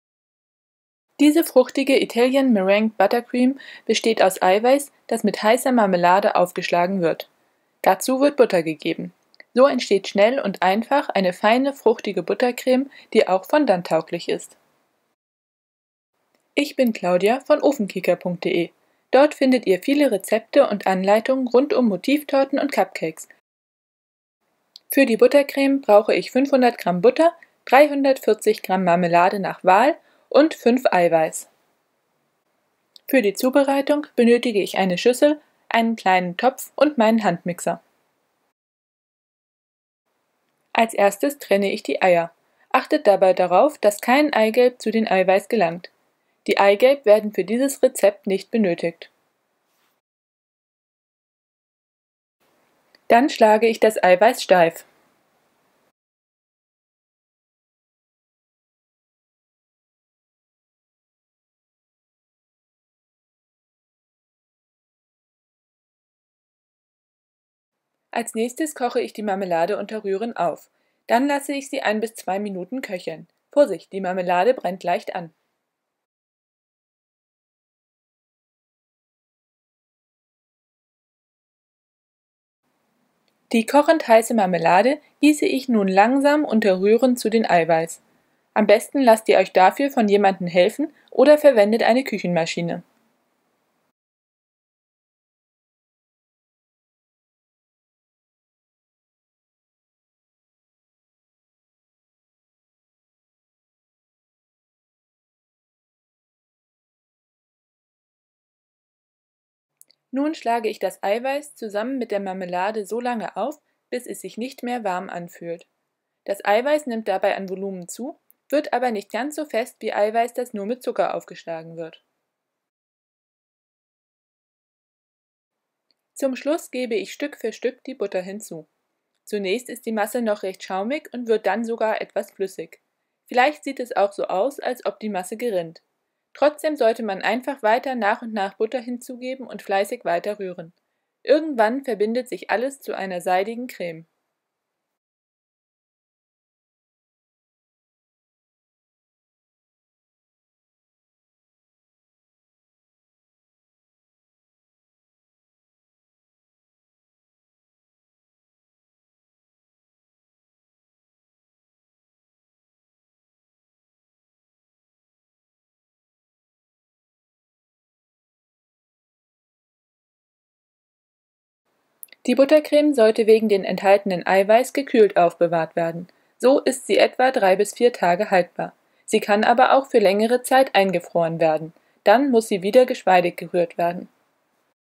Fruchtige Italian Meringue Buttercream/ fruchtige fondanttaugliche Creme
Die fruchtige Italian Meringue Buttercream wird aus Eiweiß, Marmelade und Butter hergestellt. Es entsteht eine seidige, nicht zu süße fondanttaugliche Creme. Durch die Verwendung unterschiedlicher Marmeladen lässt sich die Italian Meringue Buttercream vielfältig abwandeln.